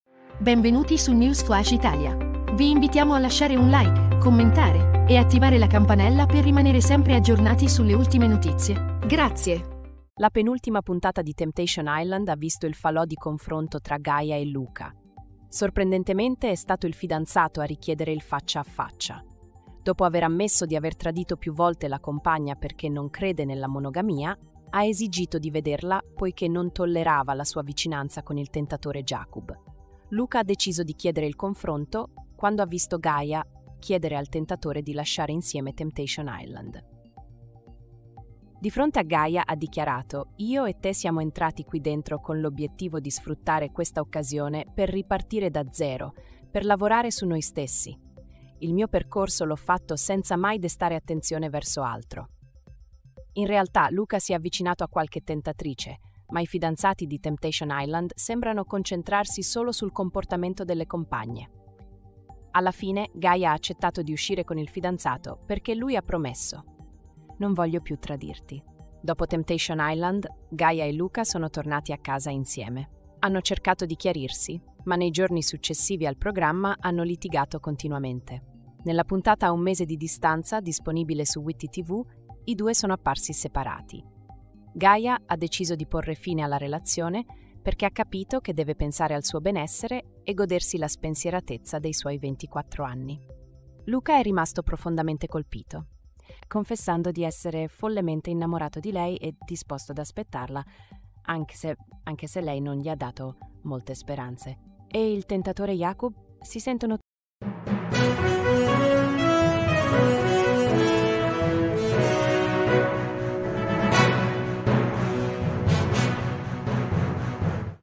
SCANDALO A TEMPTATION ISLAND: GAIA E LUCA SI SONO LASCIATI DOPO IL CONFRONTO
La penultima puntata di Temptation Island ha visto il falò di confronto tra Gaia e Luca. Sorprendentemente, è stato il fidanzato a richiedere il faccia a faccia. Dopo aver ammesso di aver tradito più volte la compagna perché non crede nella monogamia, ha esigito di vederla poiché non tollerava la sua vicinanza con il tentatore Jakub.

Luca ha deciso di chiedere il confronto quando ha visto Gaia chiedere al tentatore di lasciare insieme Temptation Island. Di fronte a Gaia, ha dichiarato: "Io e te siamo entrati qui dentro con l'obiettivo di sfruttare questa occasione per ripartire da zero, per lavorare su noi stessi. Il mio percorso l'ho fatto senza mai destare attenzione verso altro."

In realtà, Luca si è avvicinato a qualche tentatrice, ma i fidanzati di Temptation Island sembrano concentrarsi solo sul comportamento delle compagne. Alla fine, Gaia ha accettato di uscire con il fidanzato perché lui ha promesso: "Non voglio più tradirti."

Dopo Temptation Island, Gaia e Luca sono tornati a casa insieme. Hanno cercato di chiarirsi, ma nei giorni successivi al programma hanno litigato continuamente. Nella puntata a un mese di distanza, disponibile su Witty Tv, i due sono apparsi separati. Gaia ha deciso di porre fine alla relazione perché ha capito che deve pensare al suo benessere e godersi la spensieratezza dei suoi 24 anni. Luca è rimasto profondamente colpito, confessando di essere follemente innamorato di lei e disposto ad aspettarla, anche se lei non gli ha dato molte speranze.

E il tentatore Jakub? Si sentono tutti i giorni, ma per ora non si sono ancora rivisti.